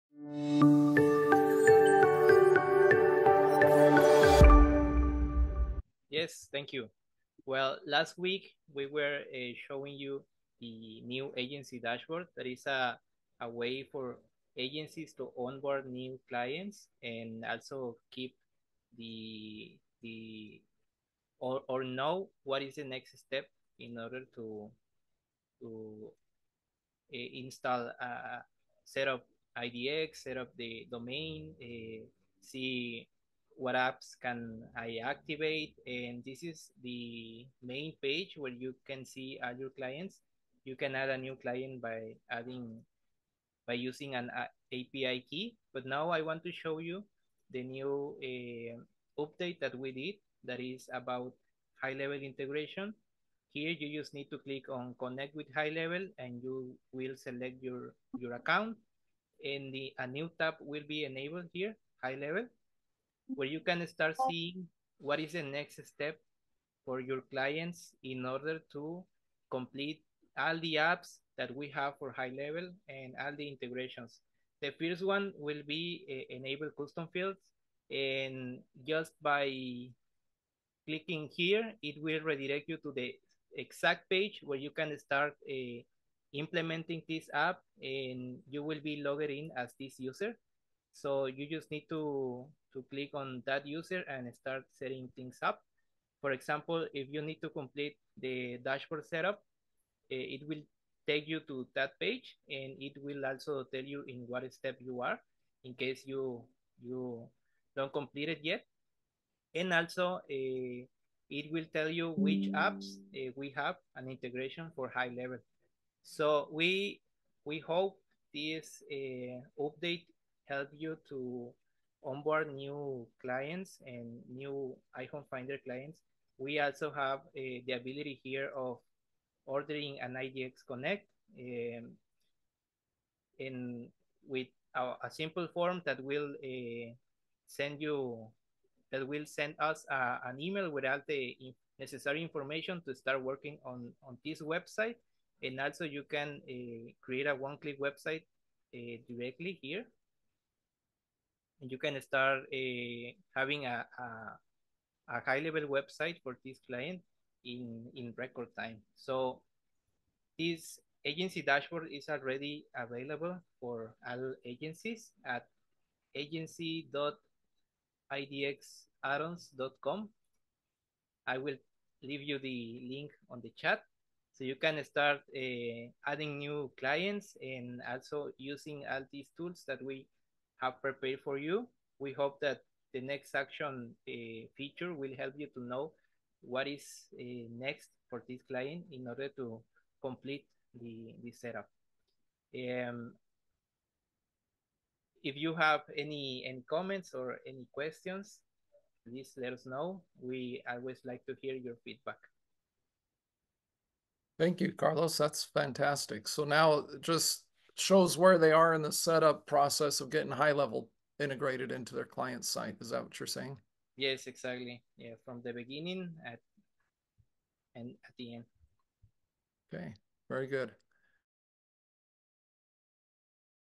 Updates for AgencyDashboard for IDX Broker and HighLevel GHL - less than 4 minutes
- New features for IDX Broker

We have added some new features to our AgencyDashboard for GHL and IDX Broker

Show which apps can be activated
Add new IDX Broker clients
Quickly integrate GHL for your sub accounts on HighLevel and setup the Dashboard.
Setup the Custom Fields
Quickly integrate GHL and IDX.